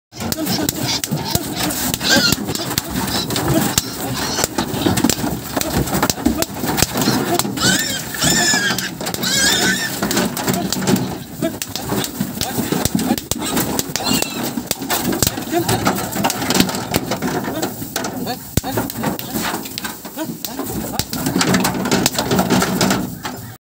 wild boar pig. we are trying to get in truck 🚛😳😱
wild war pics we are trying to get in truck but it's a difficult